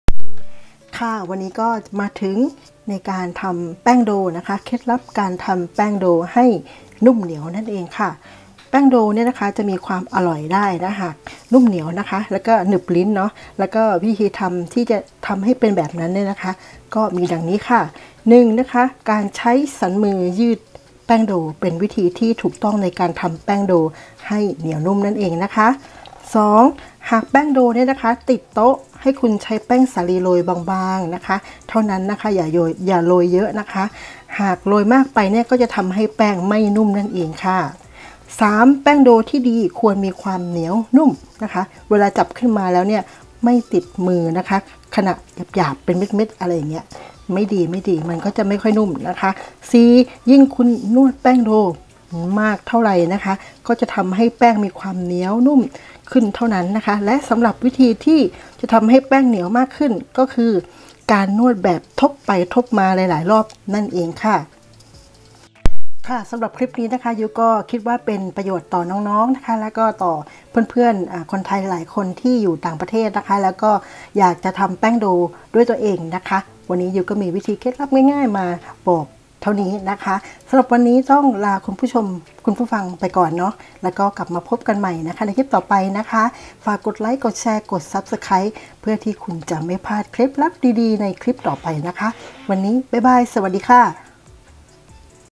เคล็ดไม่ลับ การทำ แป้งโด ให้นุ่มเหนียว | Yula Sweden
โด คือของผสมดิบที่เกิดจากการผสม แป้งข้าวสาลี และน้ำแล้วนวดให้เข้ากัน บางชนิดอาจผสมกับไขมันต่าง ๆ เช่น เนยขาว เนย หรือ เคล็ดลับ เนยเทียม ซึ่งทำให้โด เหนียว นุ่ม ยืดหยุ่น และ ดึงเป็นแผ่นบางได้โดยไม่ขาด ผลิตภัณฑ์จากแป้งข้าวสาลีที่ต้องนวดให้เกิดโด ได้แก่ ขนมปัง ซาลาเปา บะหมี่ แผ่นเกี๊ยว ปาท่องโก๋ โรตี เป็นต้น